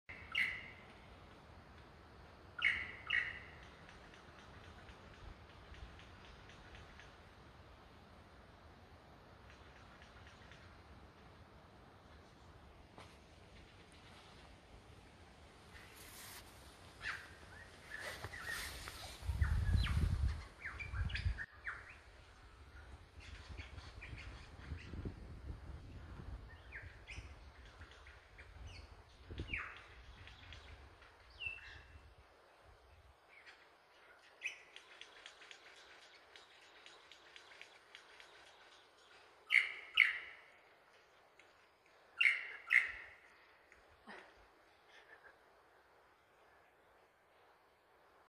Lyrebird - Menura novaehollandiae male - mating courtship display to female that passes by
Lyrebird - Menura novaehollandiae male - Caught in the mating courtship display dance 2 - Cathedral range state park, Victoria Australia birds

Female not particularly interested